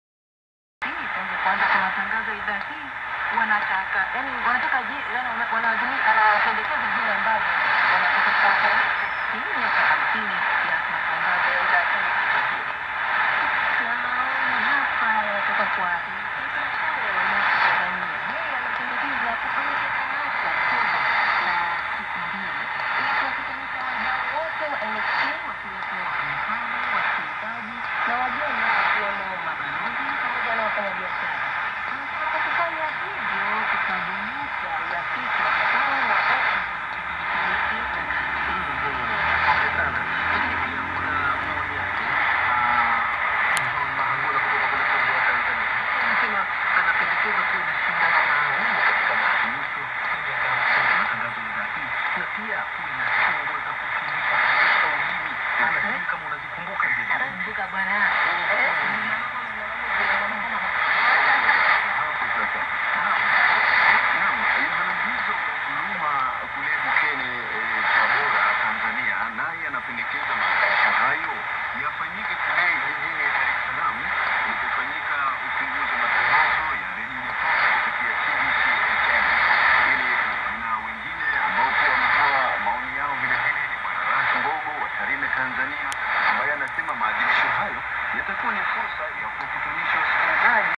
Degen DE1103 - NHK - 13730 kHz - UTC 1738 (em suaíli)
Florianópolis, 9 de fevereiro de 2014.